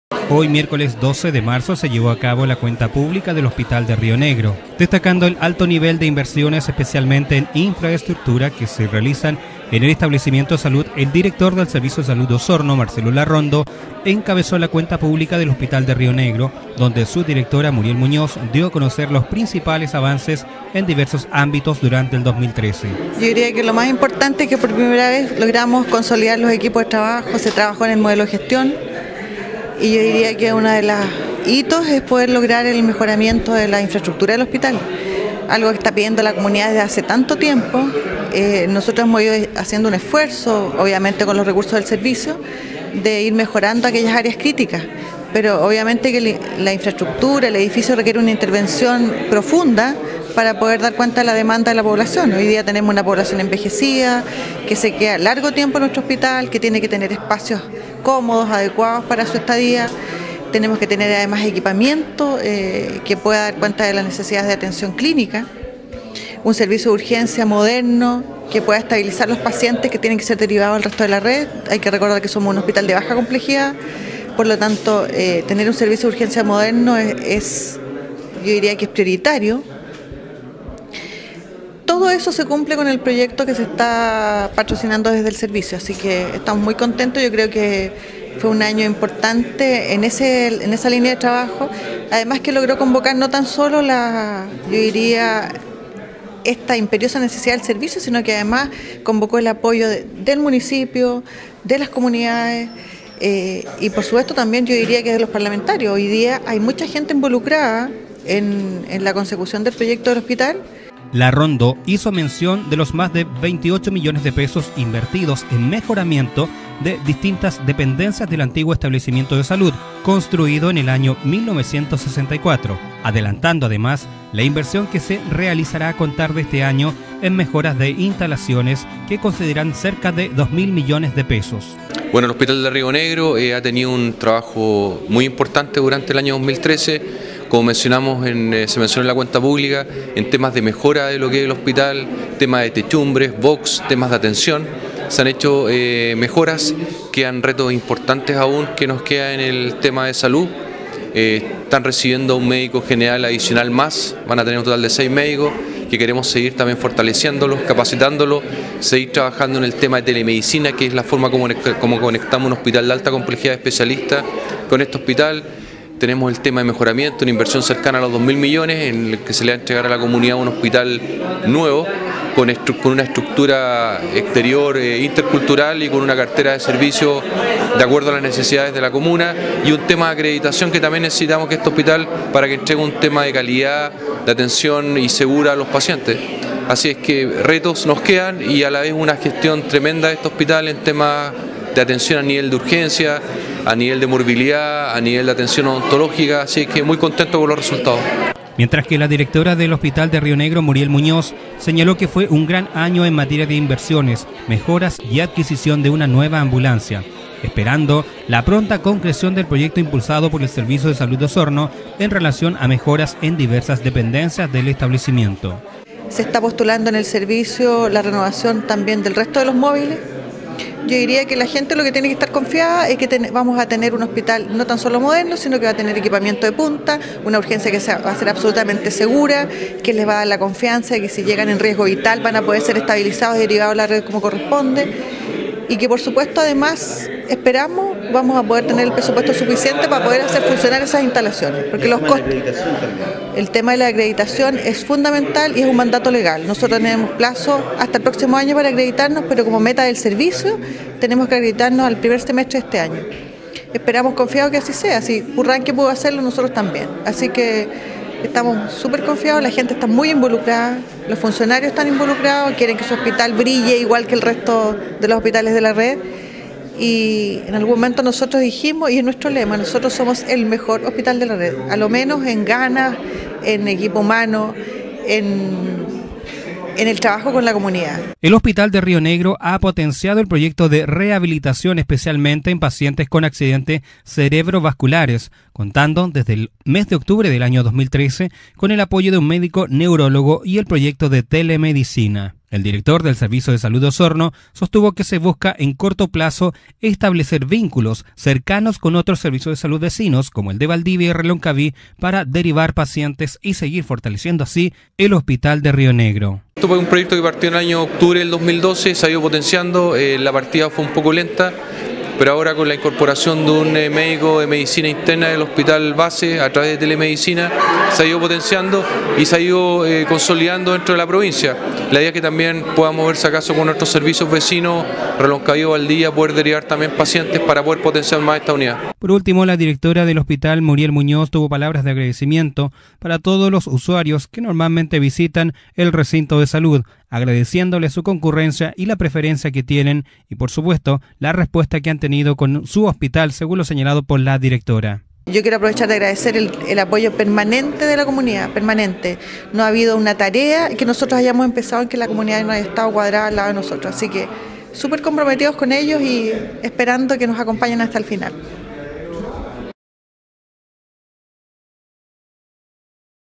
Cuenta Publica Hospital Rio Negro
En el salón azul se llevo a cabo la cuenta, donde la autoridades de Salud junto a funcionarios y el concejal Jaime Vasquez, presenciaron y aprobaron la gestión dada a conocer por la Directora.-

Destacando el alto nivel de inversiones especialmente en infraestructura que se realizan en el establecimiento de salud, el Director del Servicio de Salud Osorno, Marcelo Larrondo, encabezó la Cuenta Pública del Hospital de Río Negro, donde su Directora, Muriel Muñoz, dio a conocer los principales avances en diversos ámbitos durante el 2013.

Larrondo hizo mención a las más de 28 millones de pesos invertidos en mejoramiento de diversas dependencias del antiguo establecimiento de salud, construido el año 1964, adelantando además la inversión que se realizará a contar de este año en mejorar las instalaciones, que considera cerca de 2.000 millones de pesos.

La Directora del Hospital de Río Negro, Muriel Muñoz, señaló que fue un gran año en materia de inversiones, mejoras y adquisición de una nueva ambulancia, esperando la pronta concreción del proyecto impulsado por SSO en relación a mejoras en diversas dependencias del establecimiento.

Muñoz agregó que otro gran desafío para este año es lograr la Acreditación en Calidad, contando con el trabajo mancomunado de sus funcionarios, tal como ocurrió en el Hospital de Purranque.

El hospital de Río Negro ha potenciado el proyecto de rehabilitación especialmente en pacientes con Accidentes Cerebro Vasculares, contando desde Octubre del 2013 con el apoyo de un Médico Neurólogo y el proyecto de Telemedicina.

El Director SSO sostuvo que se busca en el corto plazo establecer vínculos cercanos con otros Servicios de Salud vecinos, como el de Valdivia y Reloncaví para derivar pacientes y seguir fortaleciendo esta unidad.